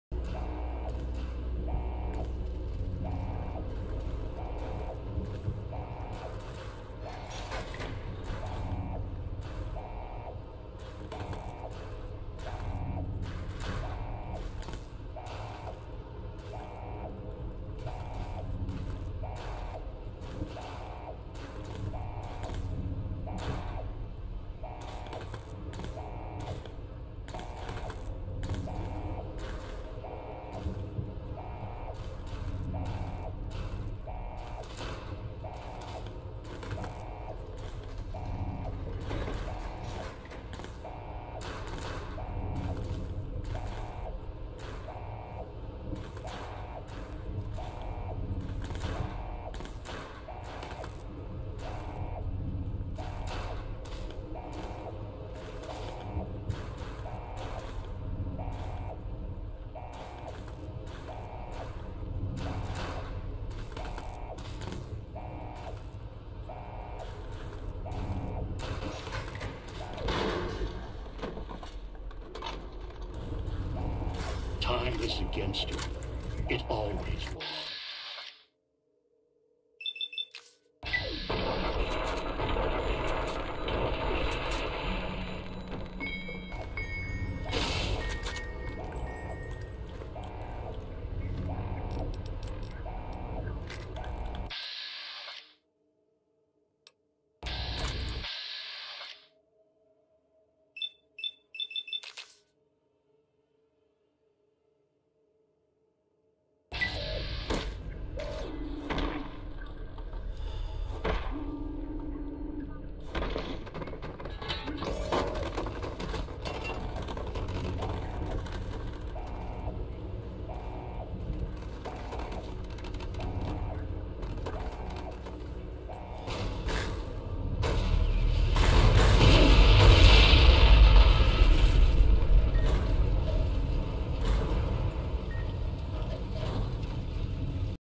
Proof that Farther Elijah is a God.
My first encounter with the god-like entity that is Farther Elijah.
After he refused to go inside the vault after I closed it on him like the silly boy he then 1 tapped me. After he received a swift fisting he teleported to another dimension too which i assume he died as the quest had been complete.